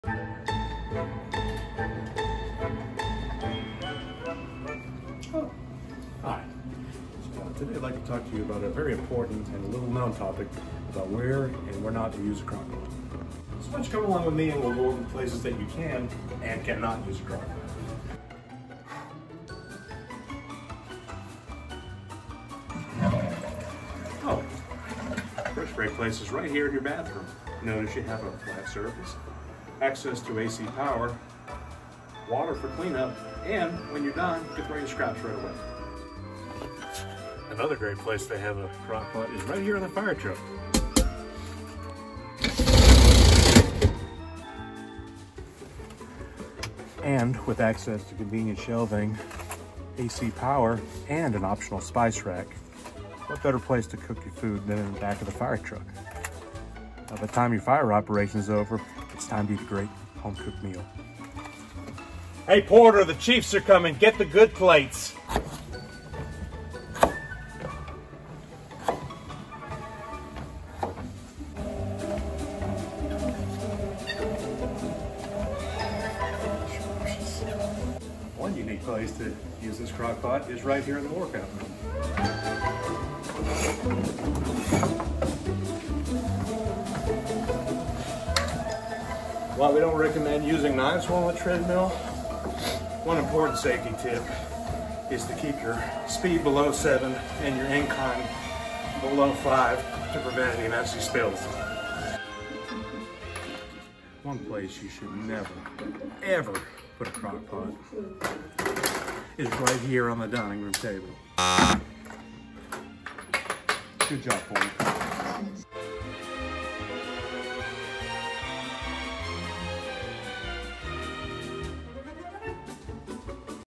Where to safely use a crockpot
Crockpot handling or miss-handling is a very serious issue. The intent of this video is to bring better awareness to crockpot use￼.